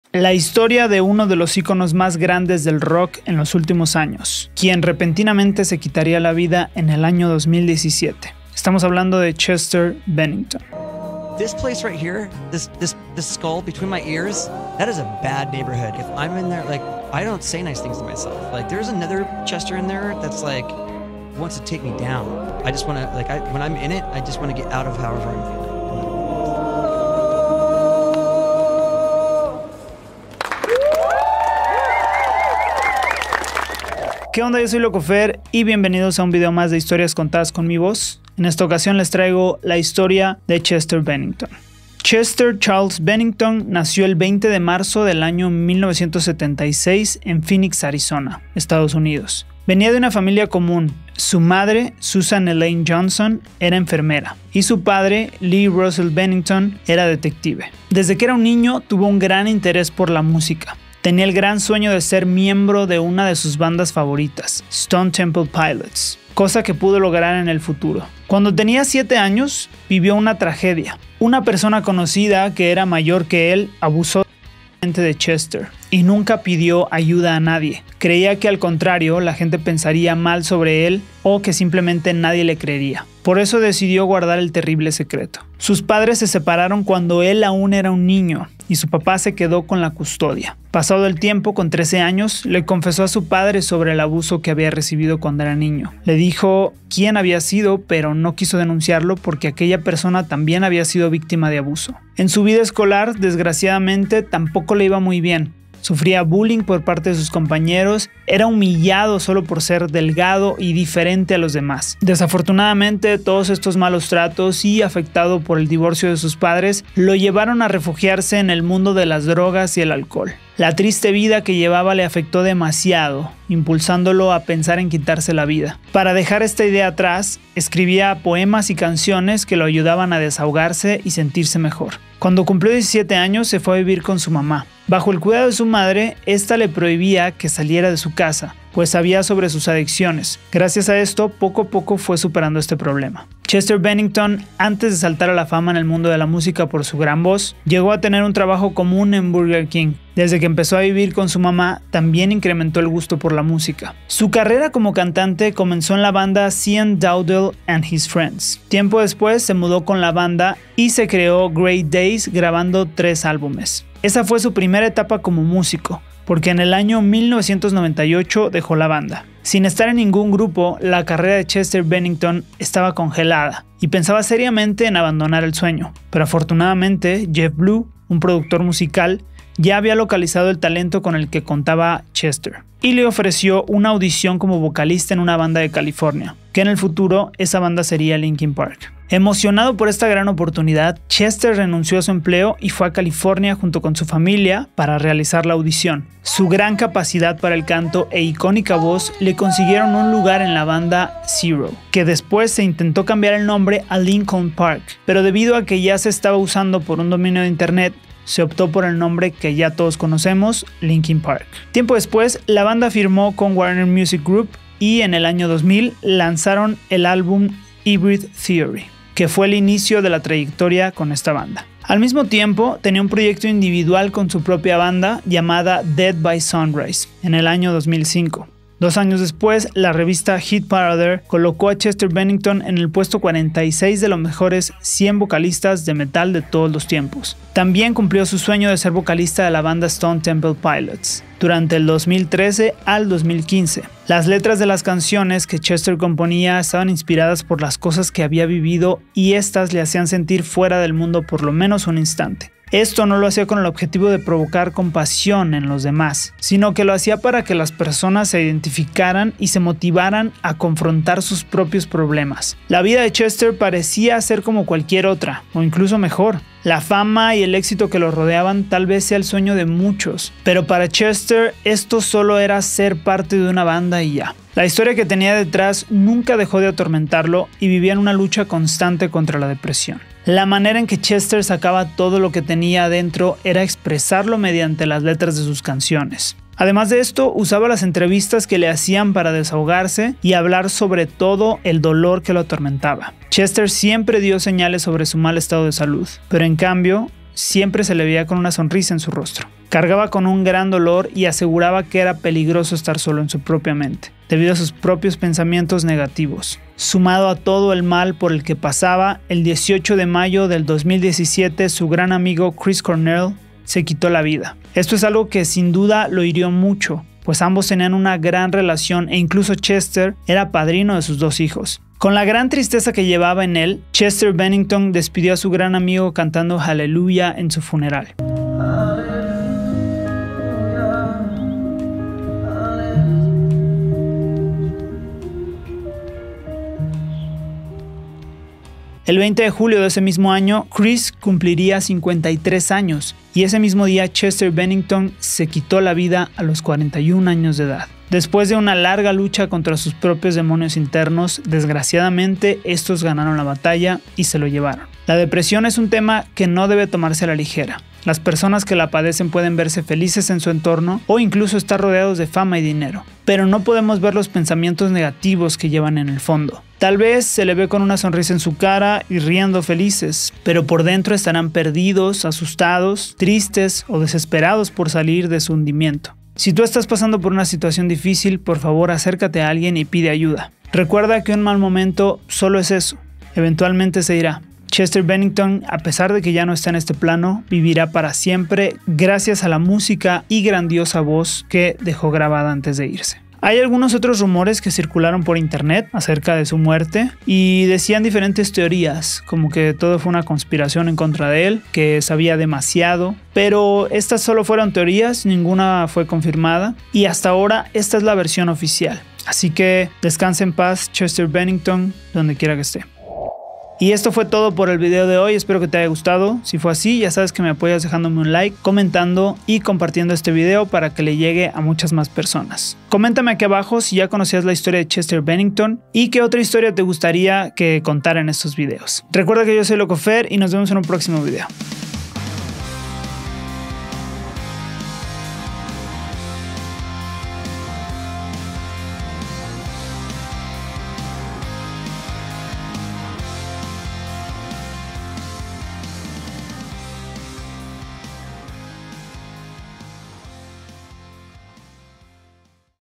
La historia de CHESTER BENNINGTON / LOCOFER
Chester Bennington fue una de las mejores voces del mundo, pero desgraciadamente vivía una lucha constante contra la depresión, lamentablemente perdió ante ella...